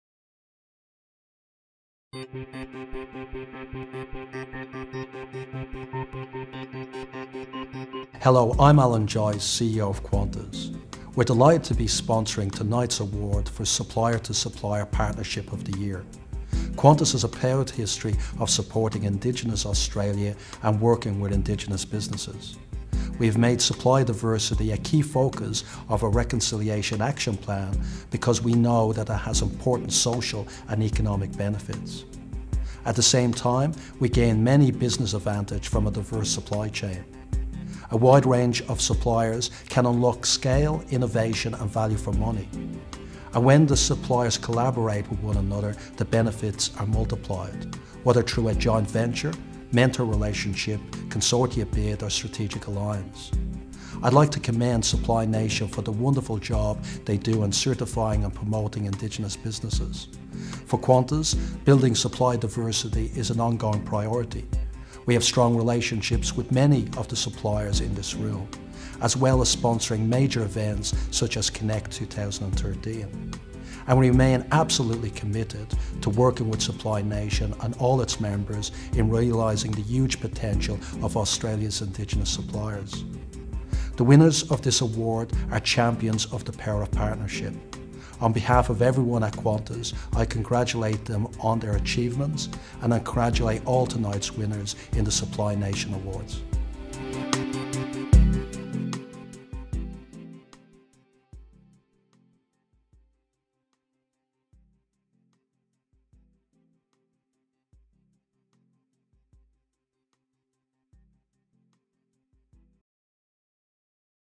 Connect 2013 Supplier to Supplier Award Sponsorship - Alan Joyce of Qantas
Supply Nation Connect Supplier Diversity Awards 2013 - Supplier to Supplier Award Sponsorship presented by Alan Joyce, CEO - Qantas.